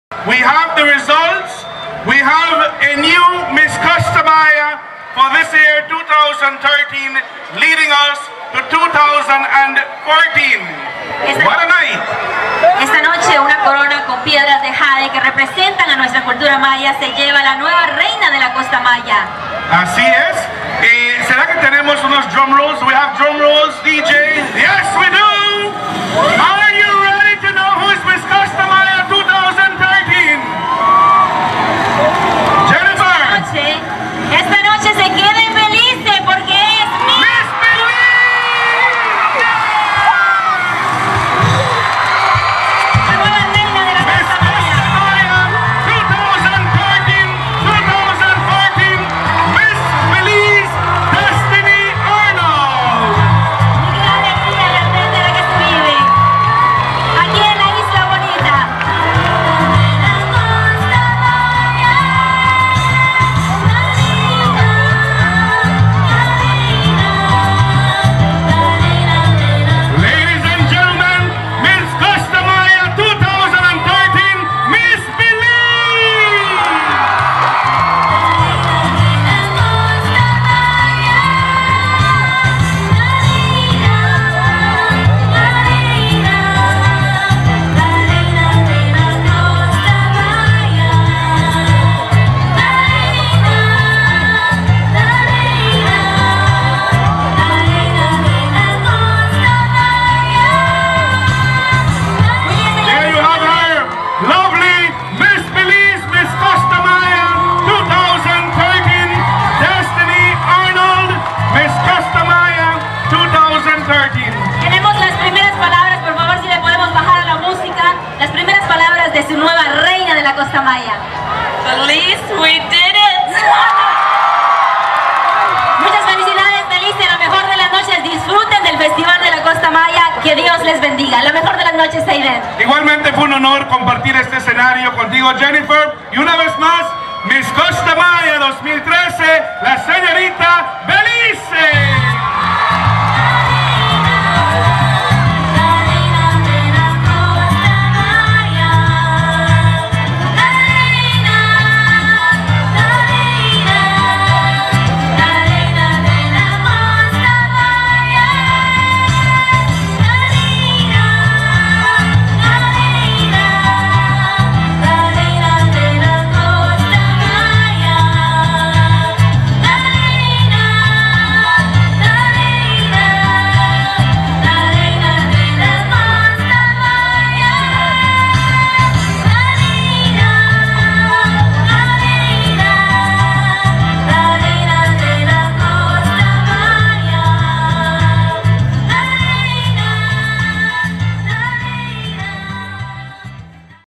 Miss Belize crowned Reina de la Costa Maya 2013, San Pedro Town, Belize
Destinee Dominque Arnold, Miss Belize, took the crown at the 2013 Reina de la Costa Maya during the International Costa Maya Festival held in San Pedro Town, Belize.